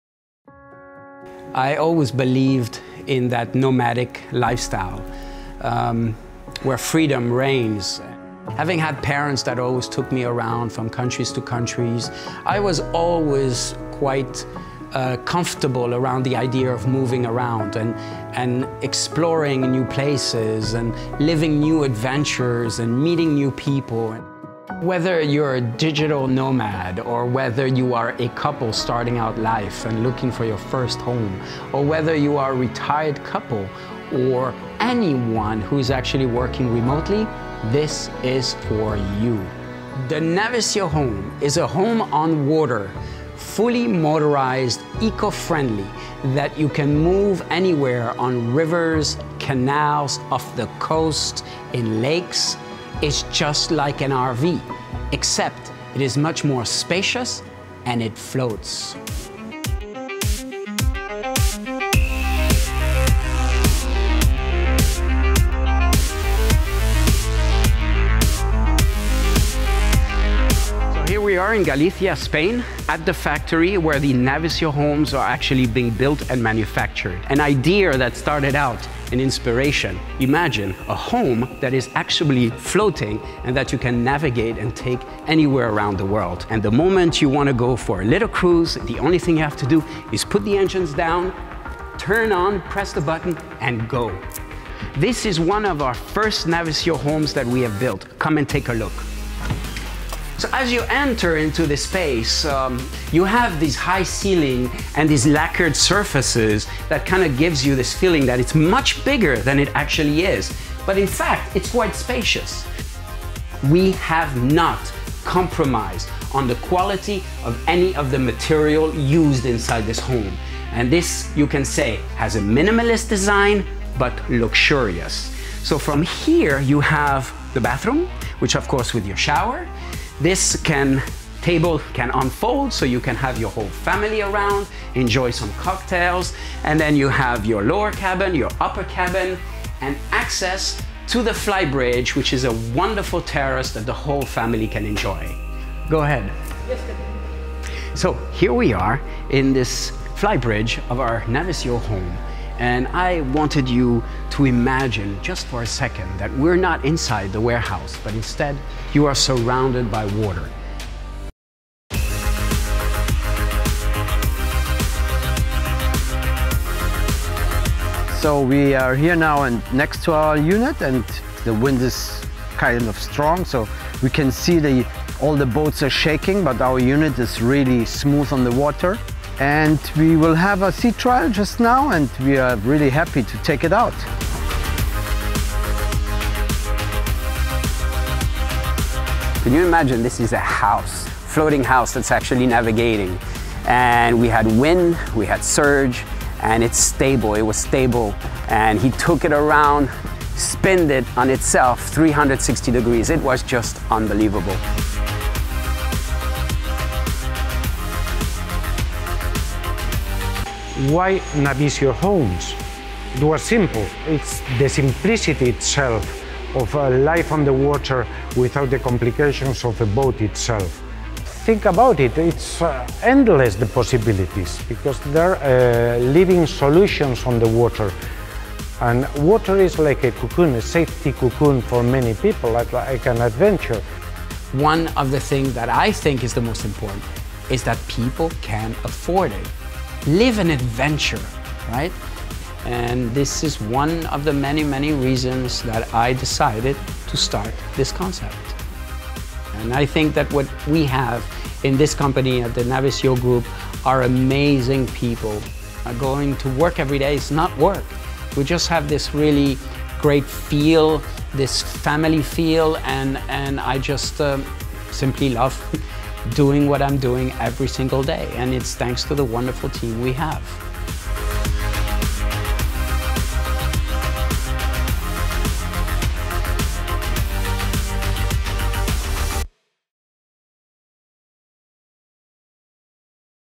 Why Navisyo Homes? | A Closer Look at Navisyo Homes Real Estate
Are you a digital nomad looking to try new adventures every single day? Or a future-savvy investor looking for the next hit?

A unique and luxurious way to invest in real estate, Navisyo Homes Real Estate offers the best of both worlds – an opportunity to own a piece of prime property and enjoy luxury cruises. See why Navisyo Homes is quickly becoming one of the most sought-after names in real estate!

This 5-minute video was shot with our entire production team at the manufacturing facility. We invite everyone around the world to watch this introductory video of our new & revolutionary motorized home on the water.

00:00 Intro
00:39 Navisyo Homes, The Home on Water
01:40 Navisyo Homes Interior
03:37 Navisyo Homes' Architectural Design
04:30 Why choose Navisyo Homes?

Navisyo Official Channel